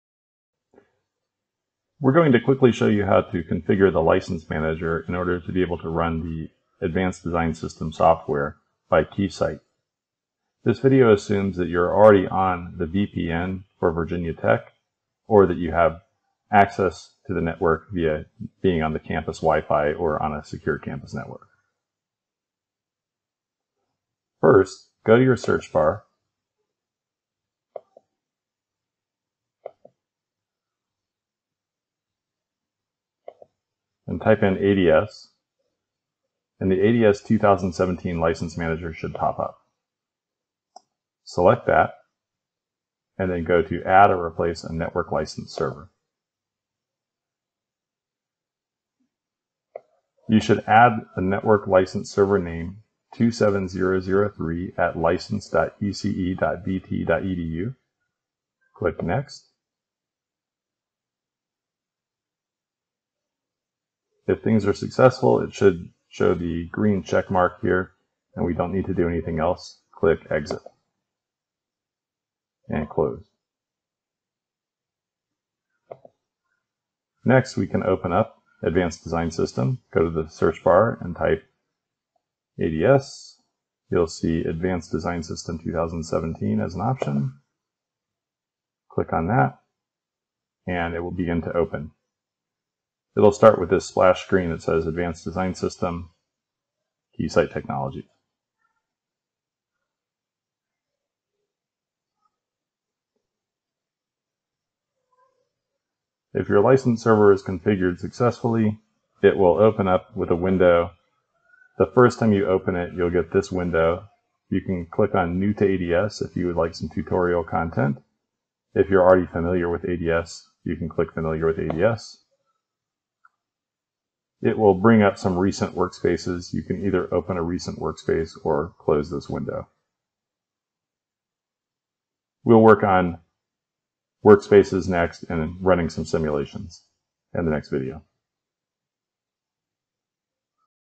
ADS - Adding License Manager
Quick video to show how to get network license manager working to run ADS at Virginia Tech. Assumes you have access to the VT network.